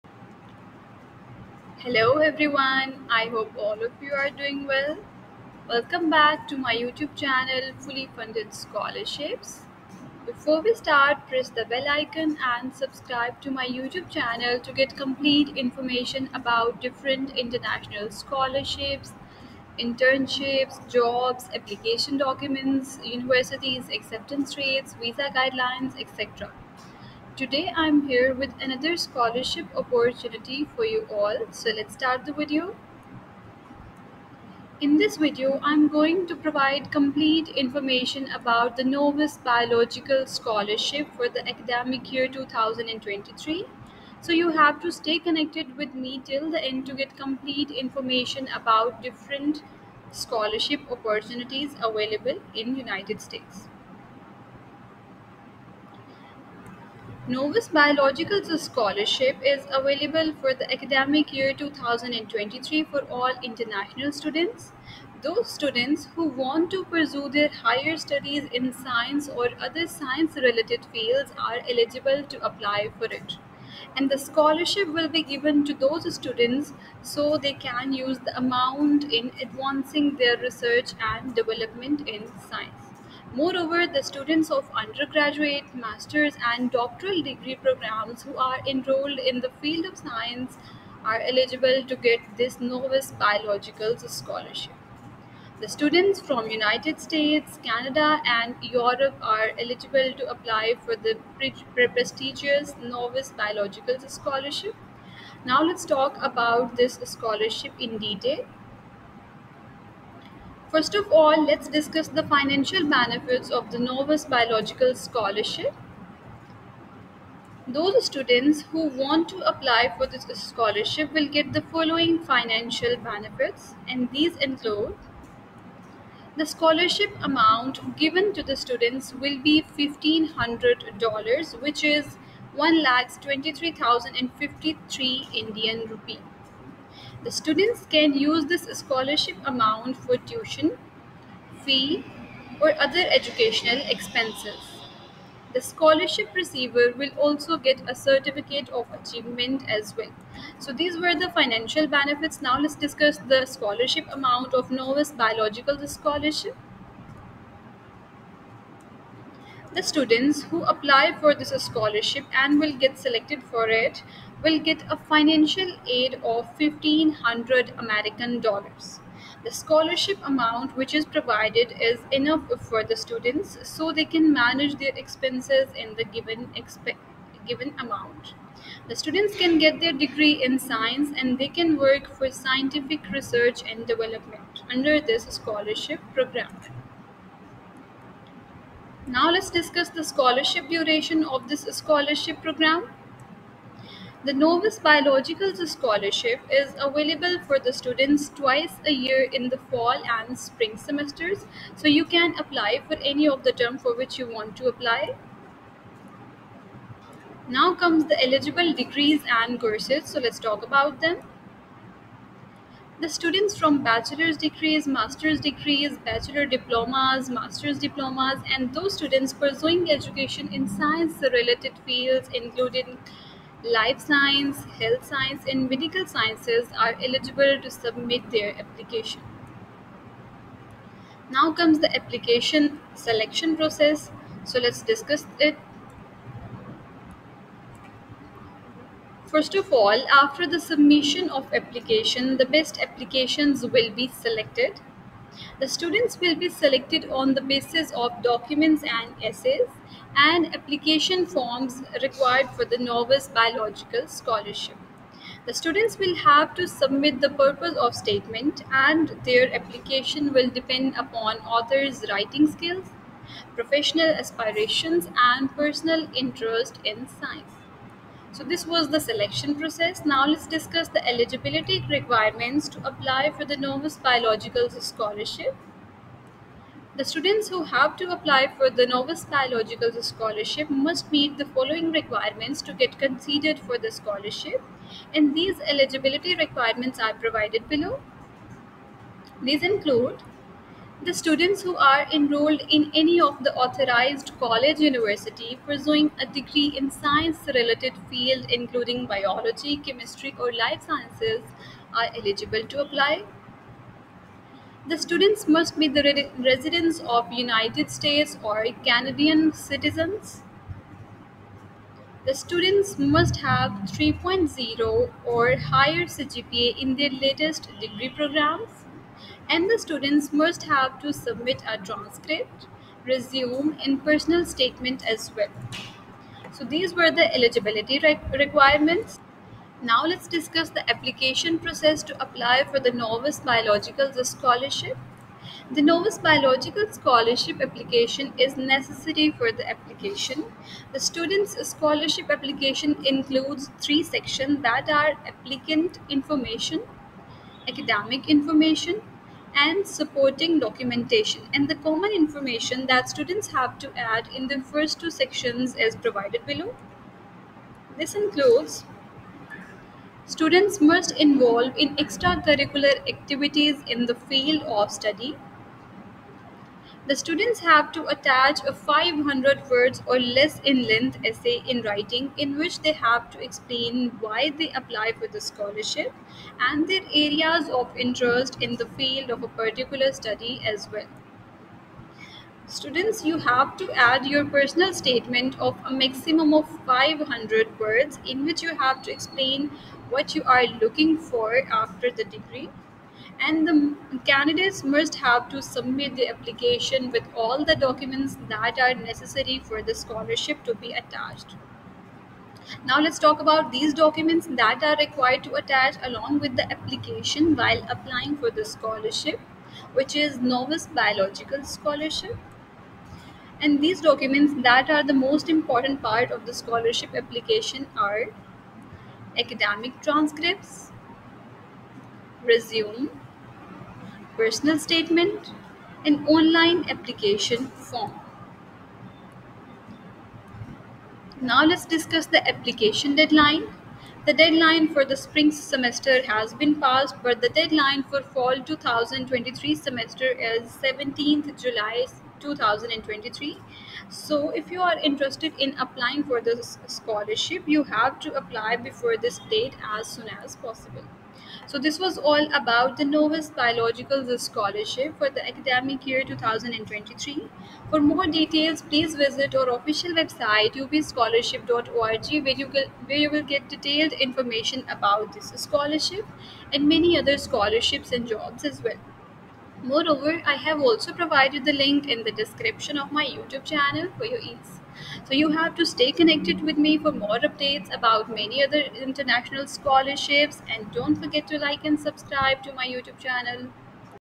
Novus Biologicals Scholarship 2023 | For Students of United States, Canada, & Europe | Apply Online
Benefits of Novus Biological Scholarship:
The scholarship amount given to the students will be $1500 (INR 1,23,053).
The students can use the scholarship amount for tuition, fees, or other educational expenses.
The scholarship receiver will also get a certificate of achievement.